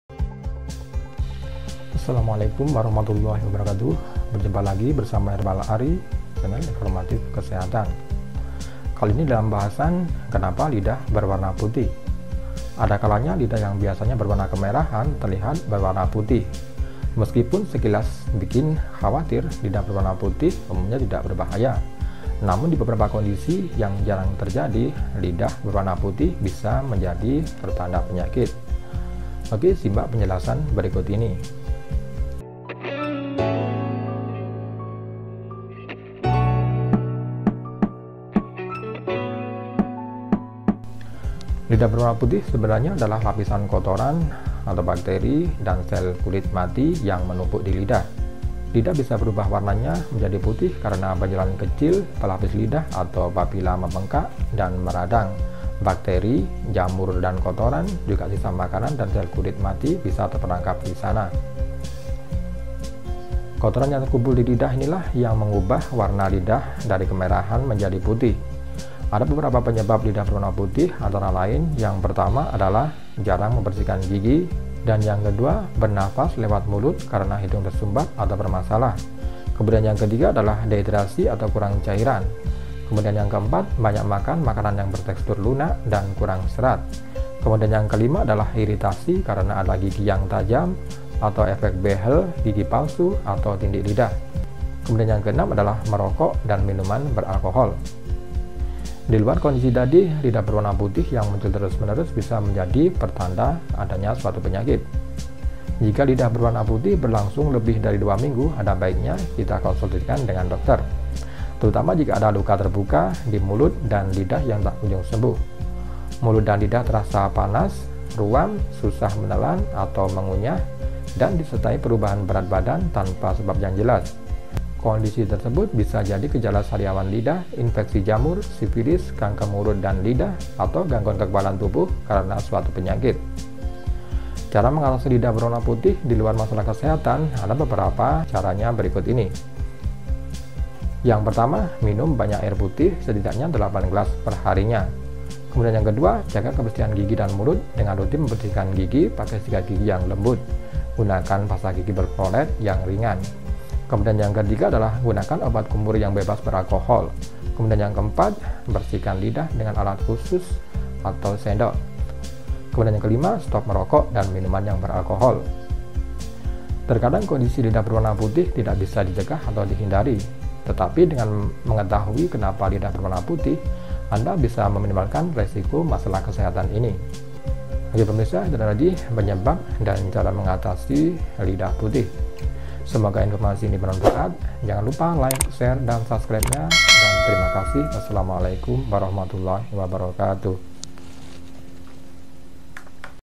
Kenapa Lidah Berwarna Putih
Ada kalanya, lidah yang biasanya berwarna kemerahan terlihat berwarna putih. Meskipun sekilas bikin khawatir, lidah berwarna putih umumnya tidak berbahaya. Namun, di beberapa kondisi yang jarang terjadi, lidah berwarna putih bisa jadi tanda penyakit